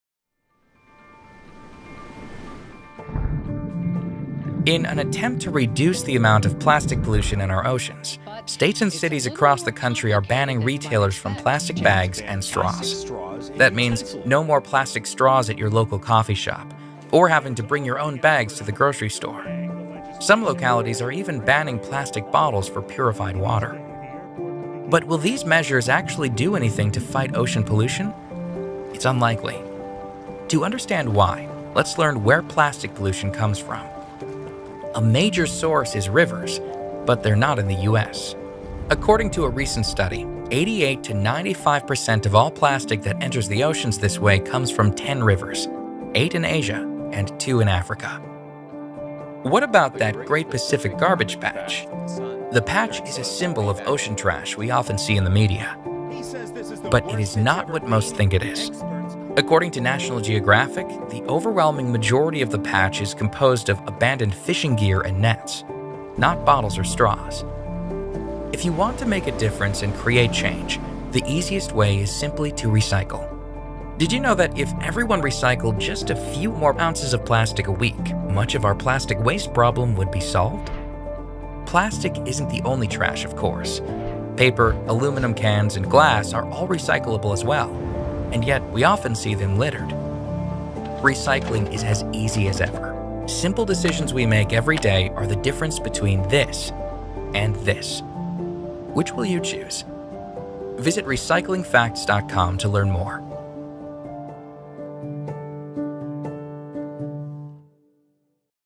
Where Does Ocean Plastic Come From?
Much of the discussion in the U.S. has focused in plastic straws. But where does plastic trash in the ocean really come from, and how can we effectively create change? The Campaign for Recycling Awareness is on a mission to share Recycling Facts.